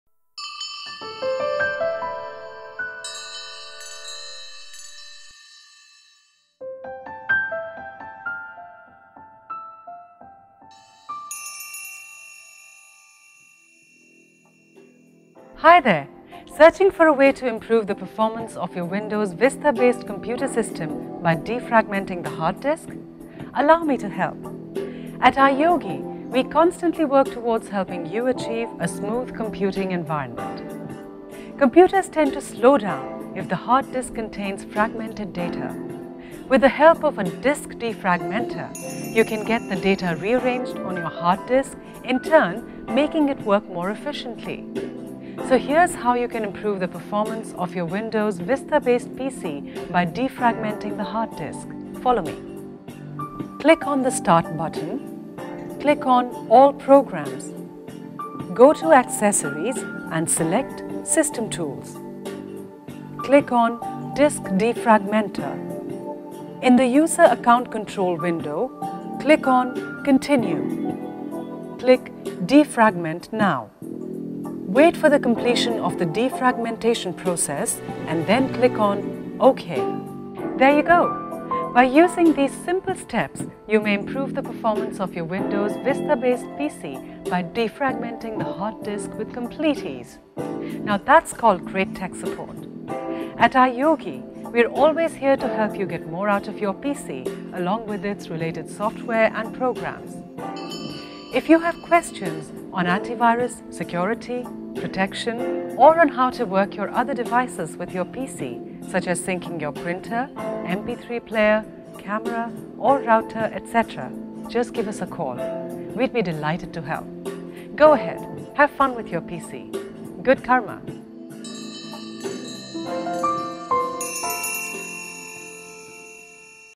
How to run Disk Defragmenter in Windows® Vista
Looking for a solution to improve the performance of your slow Windows® Vista PC? You can speed it up by defragmenting the hard disk?

Just call iYogi® (toll-free) at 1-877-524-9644 to avail the support of our tech experts.